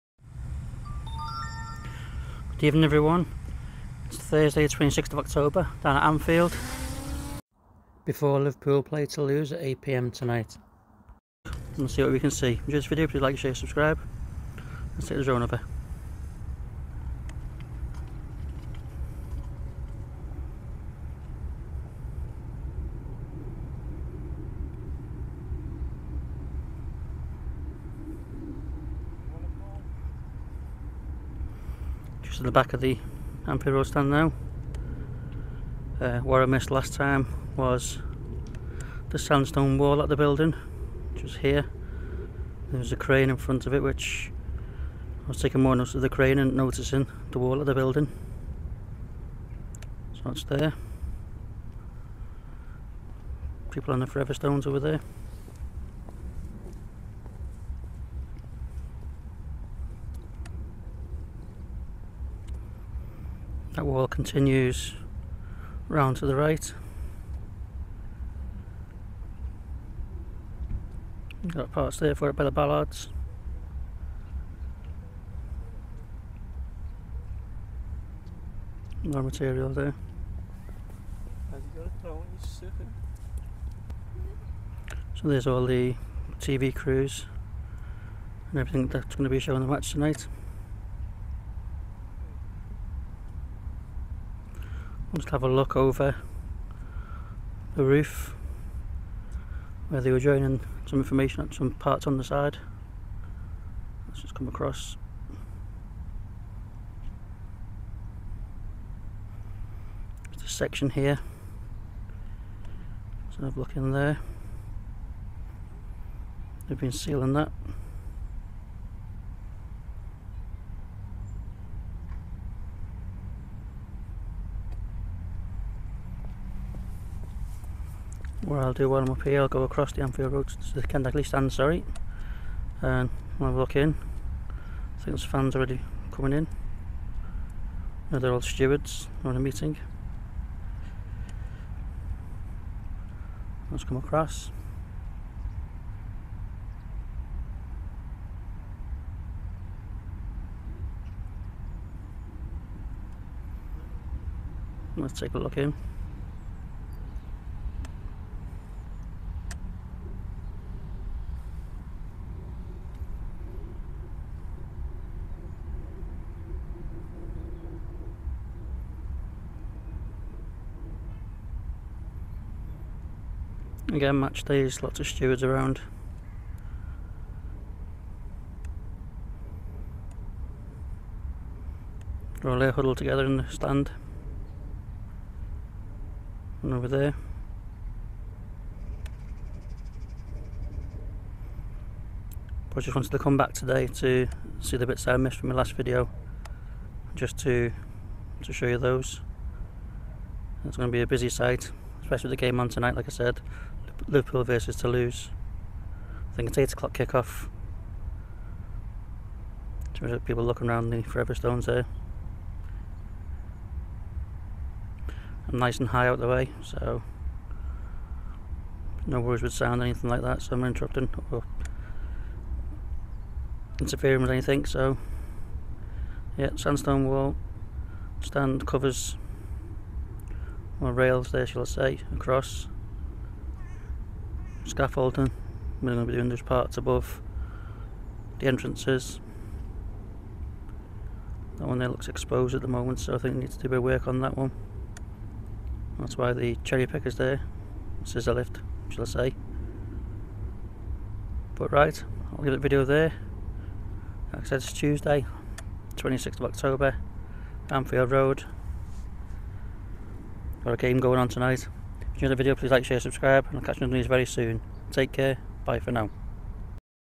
Liverpool FC Anfield Road Stand update 26-10-2023*
A look at the progress made at Anfield Road stand on Thursday 26th October. video taken approx 5.10pm before the Toulouse game. Any comments welcome. Thankyou SS Skies.